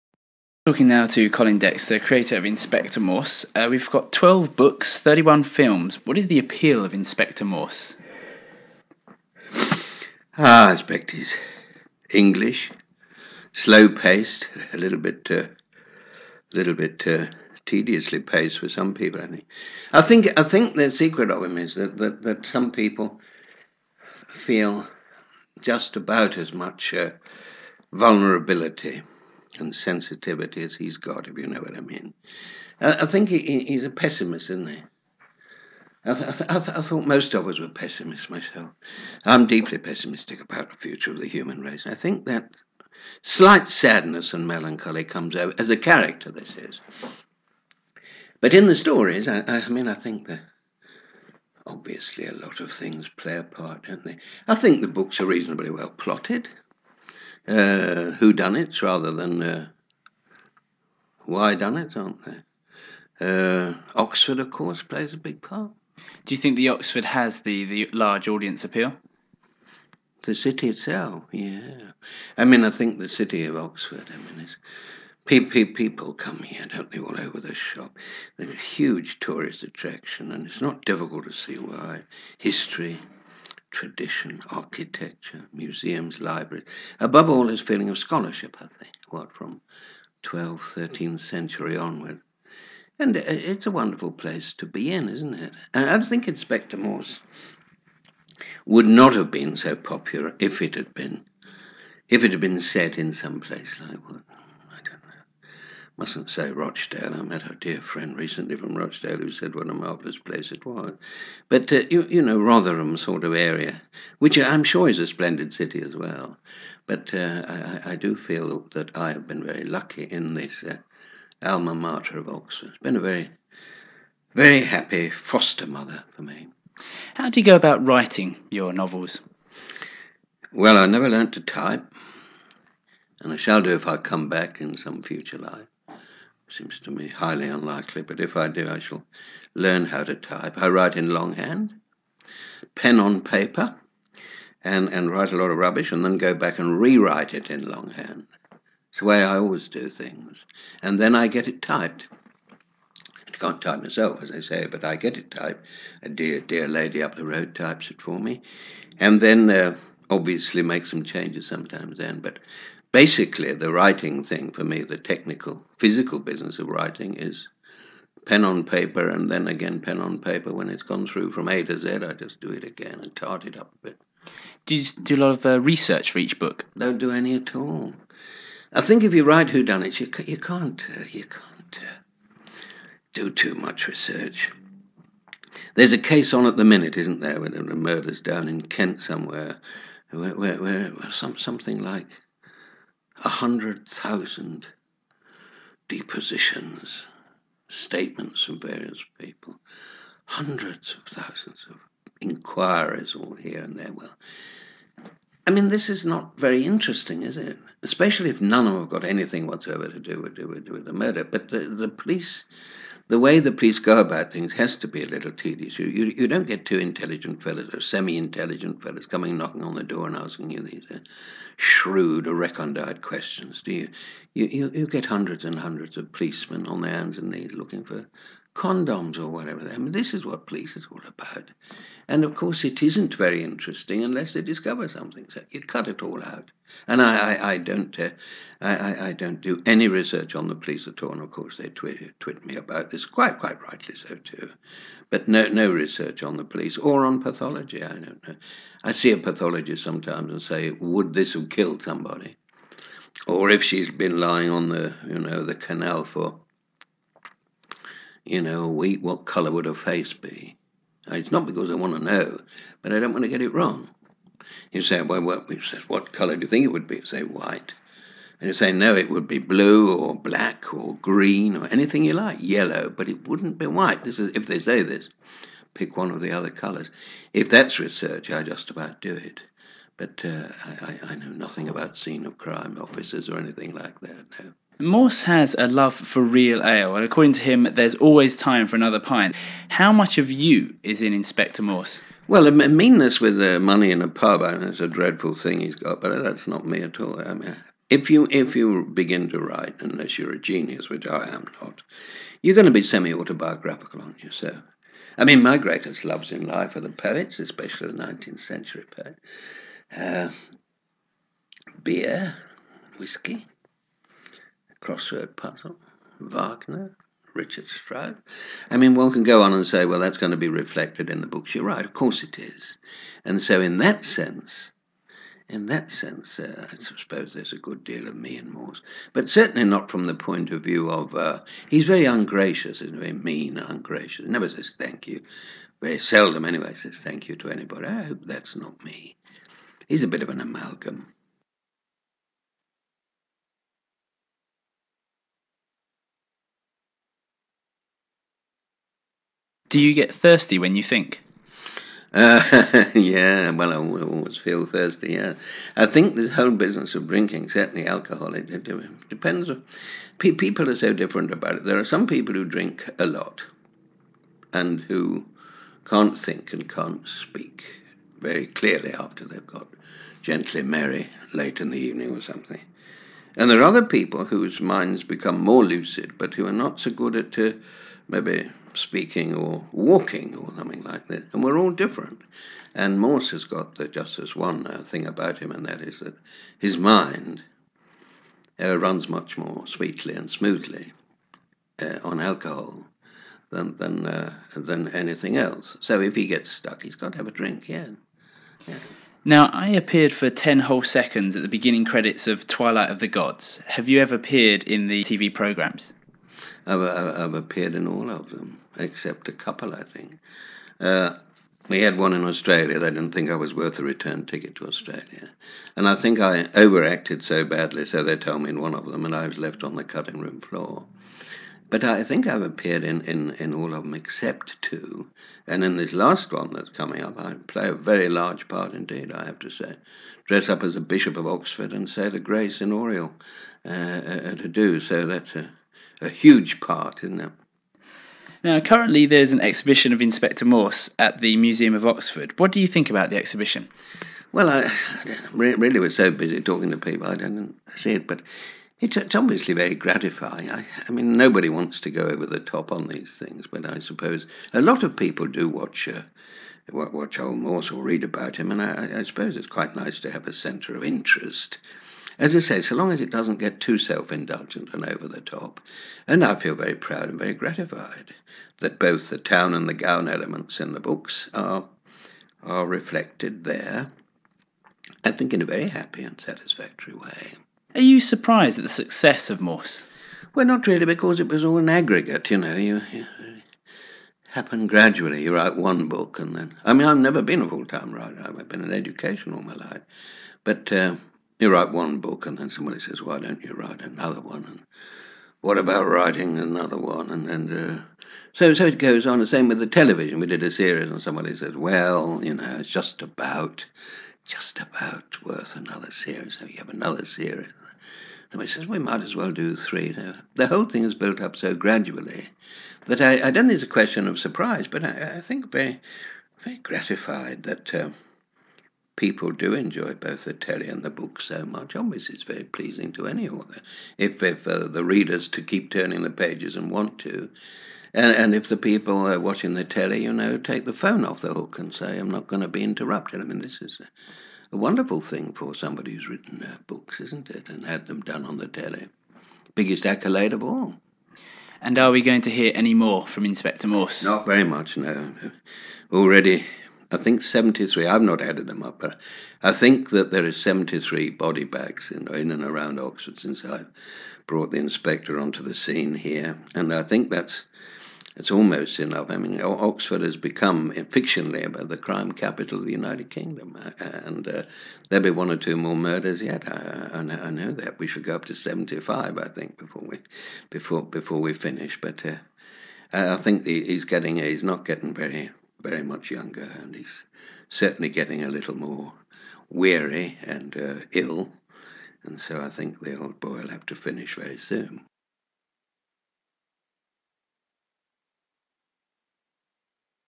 Radio Interview with Colin Dexter for Oxygen 107.9fm
Interview with Colin Dexter (writer of Inspector Morse) for radio station Oxygen 107.9fm (1996). Interview and recording by Paul Olding